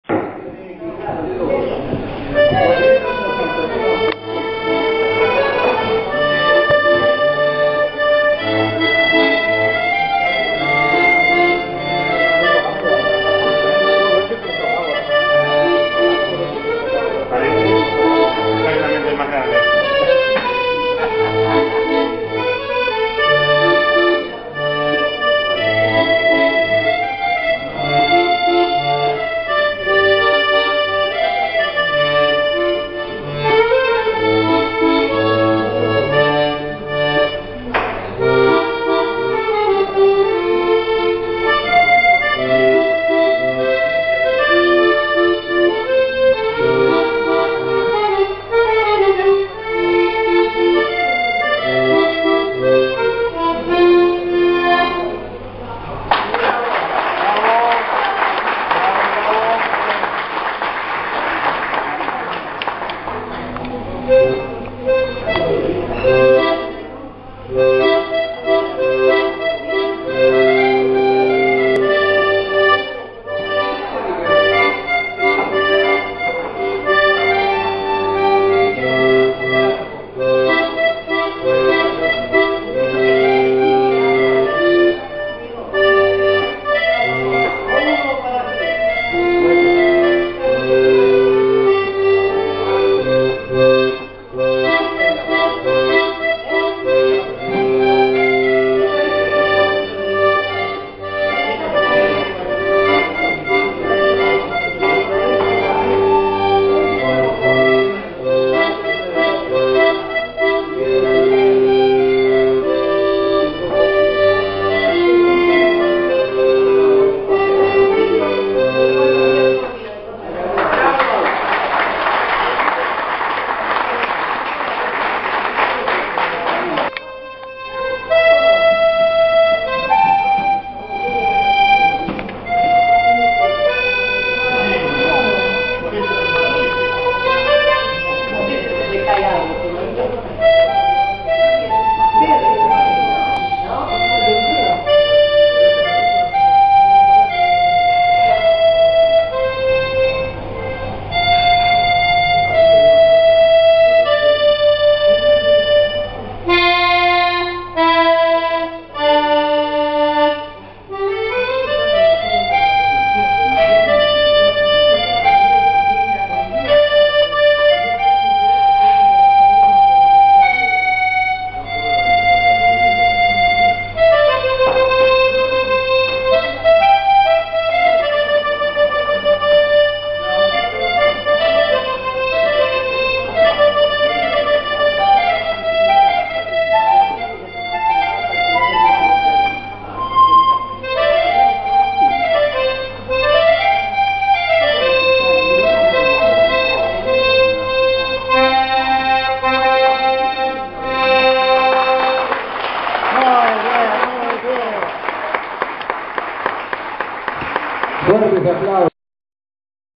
LA PEQUEÑA FIAMA Y SU REPERTORO
EN EL ENCUENTRO Nº 104 DE ACORDEONISTAS DEL DIA 26 DE OCTUBRE 2014  ,SE LE PIDE AL PUBLICO QUE RESPETE AL MUSICO QUE ESTA TOCADO GUARDANDO UN POCO DE SILENCIO.MUCHAS GRACIAS